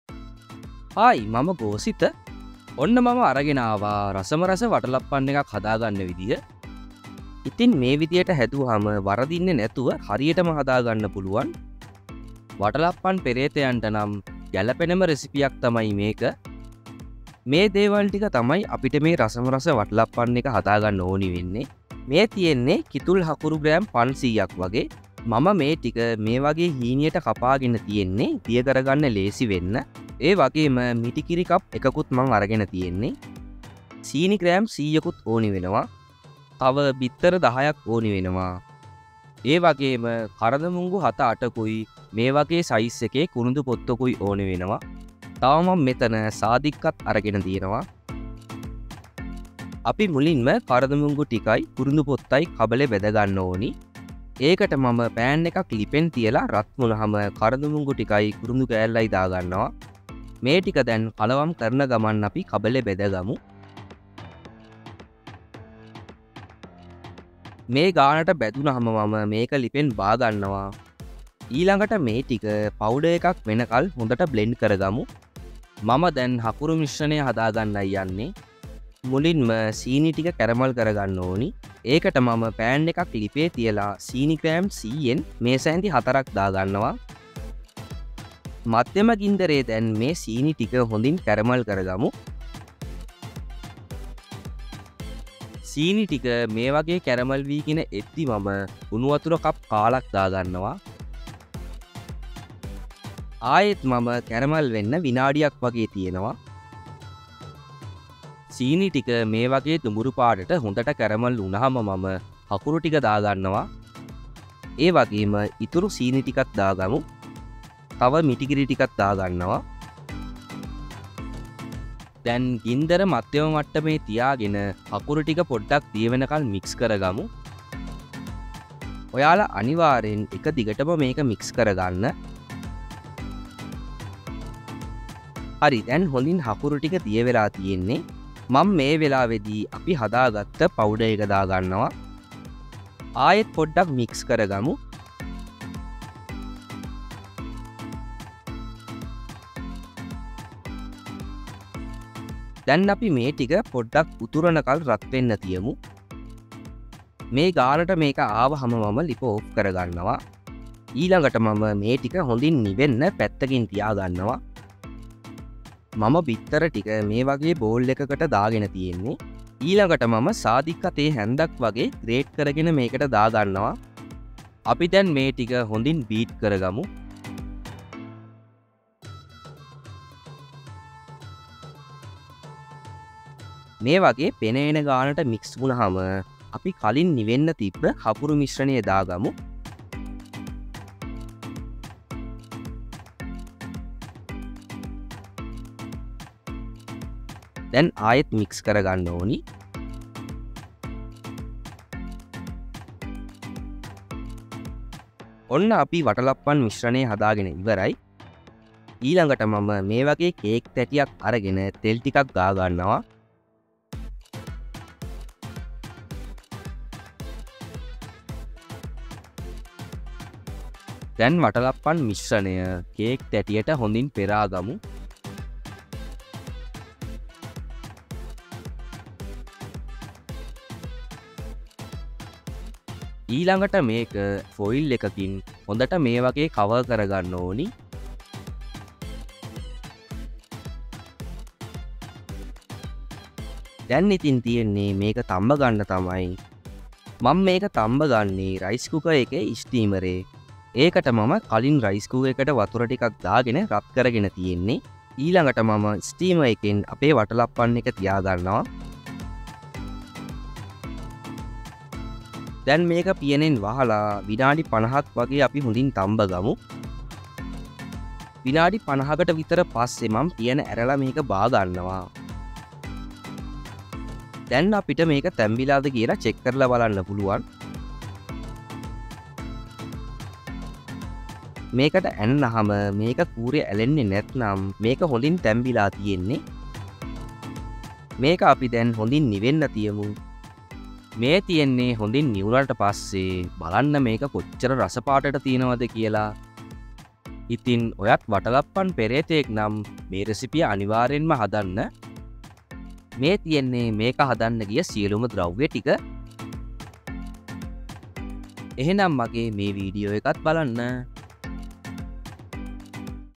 වටලප්පන් ලේසියෙන්ම රයිස් කුකර් එකේ හදමු! | Watalappan Recipe Sinhala | Sri Lanka Vatalappan
Vatalappan Recipe Sinhala | Sri Lankan Watalappan | Vatalappam in Sri Lanka

Hi, I'm Gositha, Get ready to tantalize your taste buds with a traditional Sri Lankan delight! Our Sri Lankan Vatalappan recipe is a rich, creamy coconut custard infused with aromatic spices and jaggery. Perfect for special occasions or a sweet indulgence, this dessert is sure to transport you to the tropical paradise of Sri Lanka with every bite. Let's dive into this heavenly treat and create a masterpiece that's bound to impress!

Cooking Time: 1 Hour & 20 Minutes

Ingredients:
වටලප්පන් සෑදීමට | To Make Vatalappan,
• කිතුල් හකුරු, ග්‍රෑම් 500 | Kithul Jaggery, 500 Grams
• සීනි, ග්‍රෑම් 100 | Sugar, 100 Grams
• උකු පොල් කිරි, කප් 1 (පොල් බෑයක) | Thick Coconut Milk, 1 Cup
• බිත්තර, 10 | Eggs, 10
• සාදික්කා, තේ හැදි 1 | Nutmeg, 1 Tsp
• කරදමුංගු, 8 | Cardamom, 8
• කුරුදු පොතු, පොඩි කෑල්ලක් | Cinnamon, A Small Piece
• උණු වතුර, කප් ¼ | Hot Water, ¼ Cup

හායි, මම ගෝසිත. වටලප්පන් හරියටම හදන විදිහ තමයි අද මම අරගෙන ආවේ. මාරම රසට පැණි බේරෙන වටලප්පම් එකක් තමයි මේ. ඉතින් ඔයත් වටලප්පම් පෙරේතයෙක්නම් මේ විදිහට හදන්න අනිවාර්‍යෙන්ම.

Easy Watalappan | Watalappan in Sinhala | How to Make Vatalappan | වටලප්පන් | වටලප්පම්

Chapters,
Intro 0:00
Ingredients 0:17
Making the Powder 0:54
Making Jaggery Mixture 1:17
Preparing Vatalappam Mixture 2:45
Making Vatalappan 3:28
Preserving 4:50